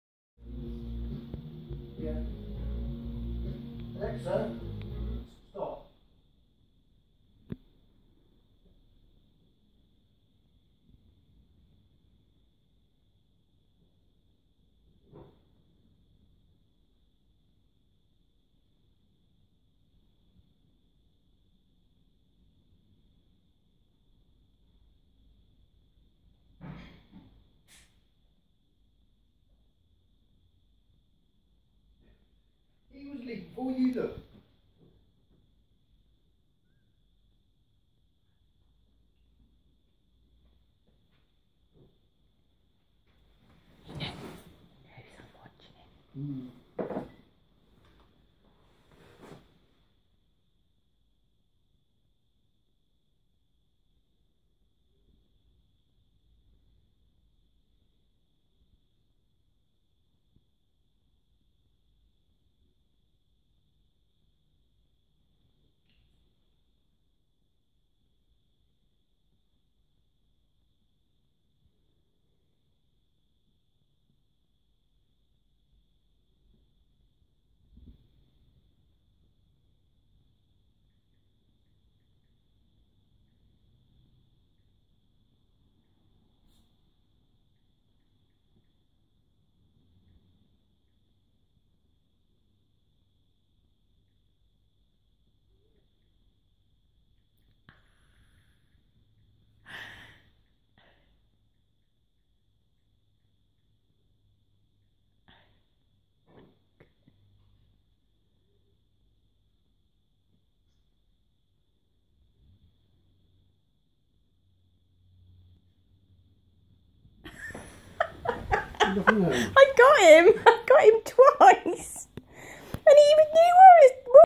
Labrador steals apples from tree
My dog loves apples, he even takes the best ones from the tree in our garden.